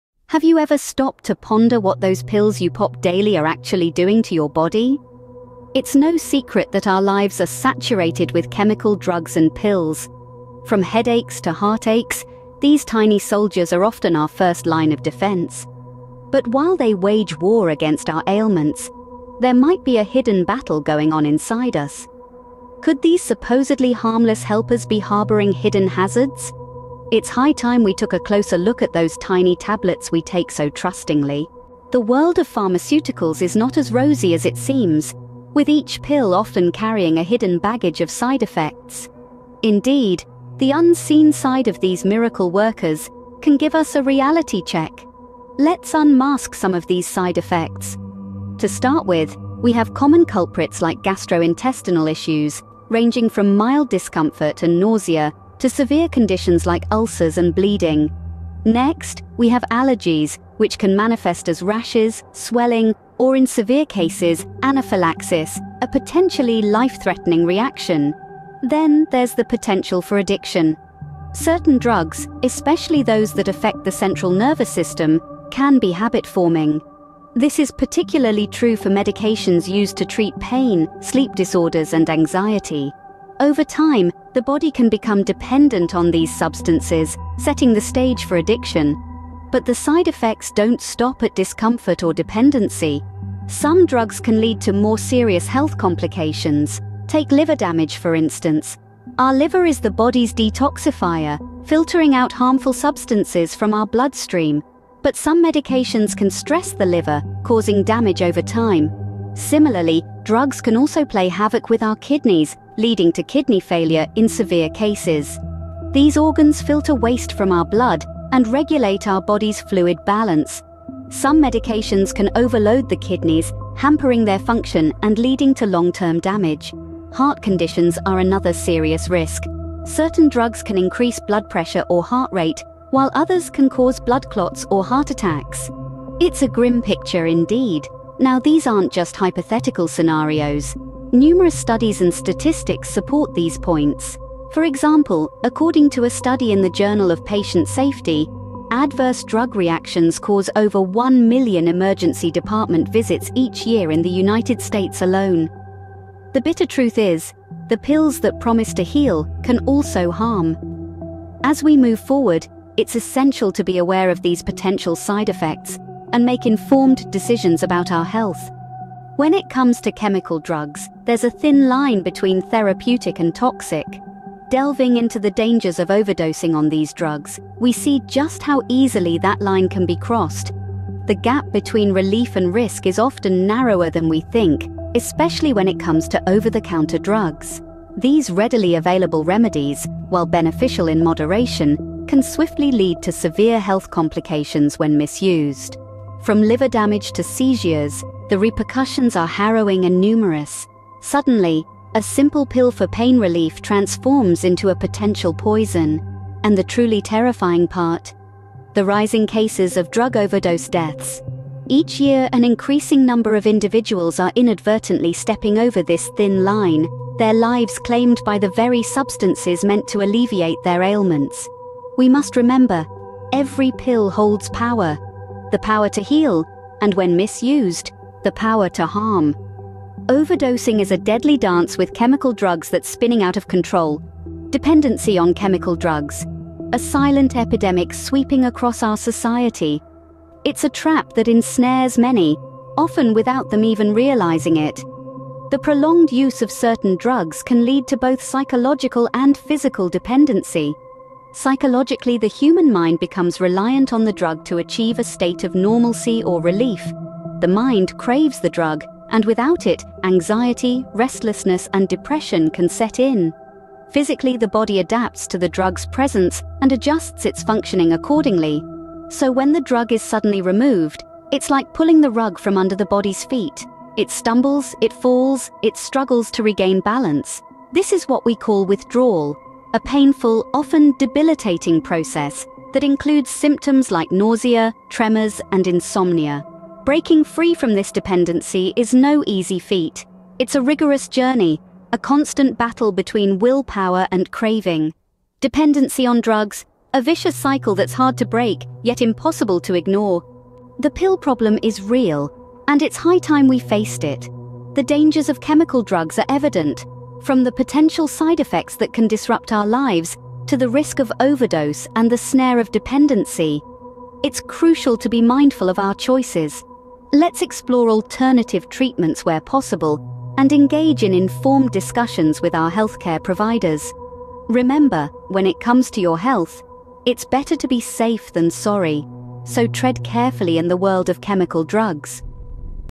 Unveiling the Hidden Dangers: The Harms and Side Effects of Chemical Drugs
In this eye-opening video, we delve into the often overlooked risks and consequences of chemical drugs on our health and well-being. From common over-the-counter medications to prescription pharmaceuticals, the use of chemical drugs has become ubiquitous in modern society. However, behind their promise of relief lies a dark underbelly of potential harm.

Join us as we shine a light on the adverse effects that chemical drugs can have on the body, mind, and overall quality of life. From debilitating side effects to long-term health complications, we explore the myriad ways in which these substances can wreak havoc on our systems.

Discover the importance of informed decision-making when it comes to managing your health and explore alternative approaches to wellness that prioritize natural remedies and holistic healing modalities. From herbal supplements to lifestyle interventions, we provide actionable insights to help you navigate away from the dangers of chemical drugs and toward a healthier, more sustainable path to well-being.

Whether you're a healthcare professional, a concerned individual, or someone seeking answers about the medications you're currently taking, this video is a must-watch. Empower yourself with knowledge and take control of your health destiny today.